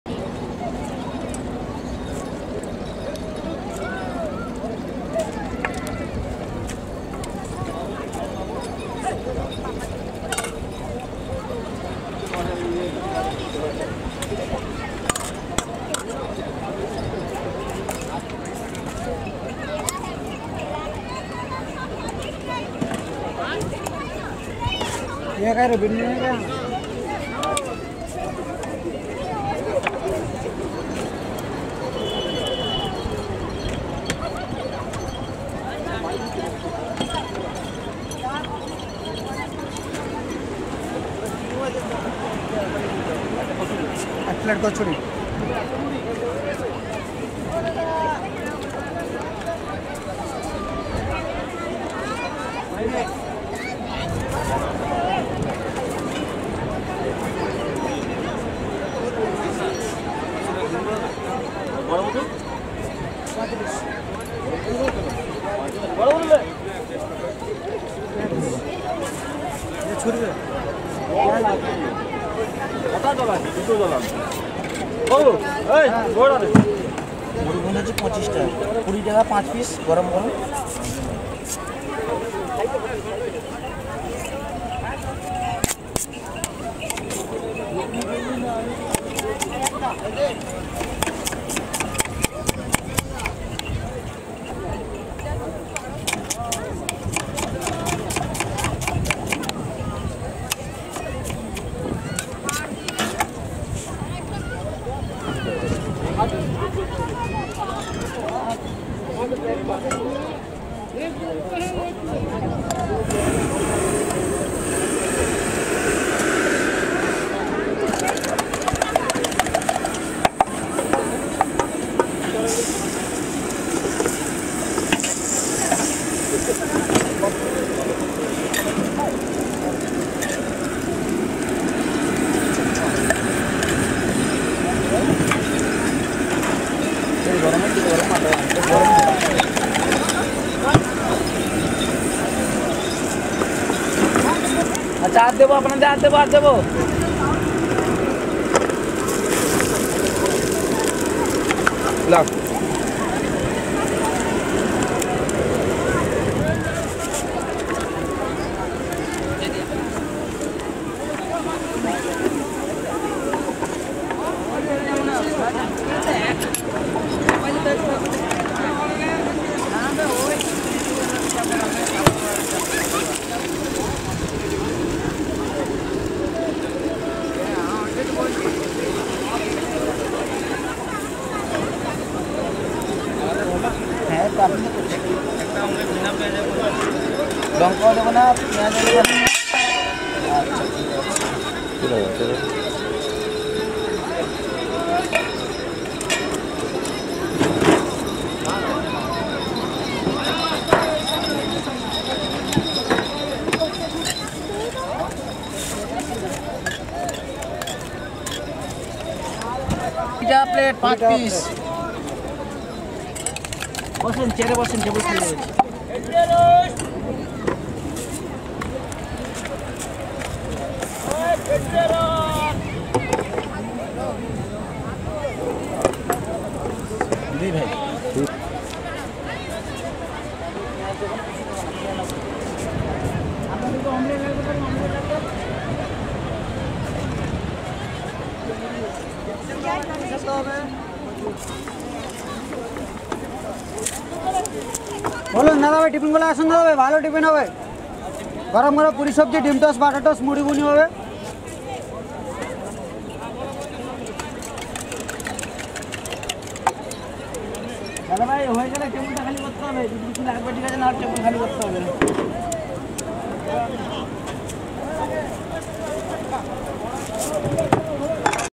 Famous Muri Ghugni 25 rs & Egg Toast 20 rs | Digha Sea Beach | Indian Street Food
Address : Digha Mohona Sea Beach , West Midnapore , West Bengal , India .